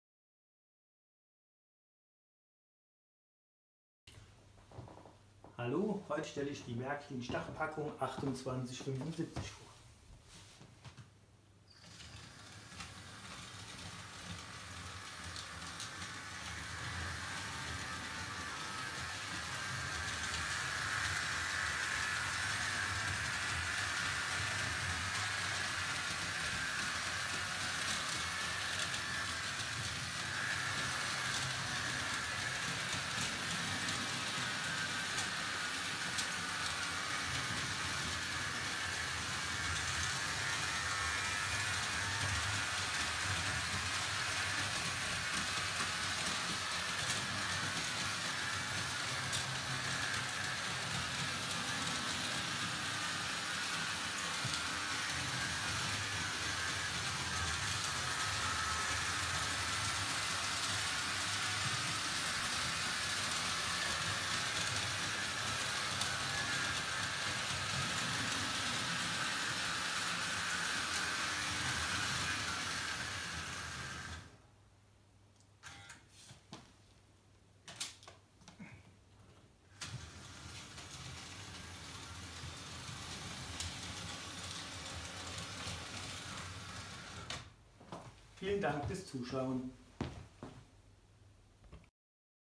Selten! Großer Güterzug Startpackung 2875 Märklin H0 analog Vintage - Eisenbahn spielen
Die analoge Startpackung hat folgenden Inhalt:

1 Diesellok  3072,
1 gedeckter Güterwagen 4411,
1 offener Güterwagen 4431,
1 Kesselwagen 4442,
1 Niederbordwagen 4474,
1 Autotransportwagen 4613 inkl. 2 Porsche        und 2 Käfer,
12 gebogene Gleisstücke 5100
19 gerade Gleisstücke 5106
1 Anschlussgleisstück,
1 Paar Weichen 5202
1 Weiche 5202 rechts
2 gebogene Gleisstücke
1 Doppelkreuzungsweiche 5207
1 Stellpult 7072
3 Prellböcke 7190
1 Verteilerplatte 7209
Kabel, Stecker und Muffen

Es handelt sich um eine Startpackung aus den 70-/80er Jahren.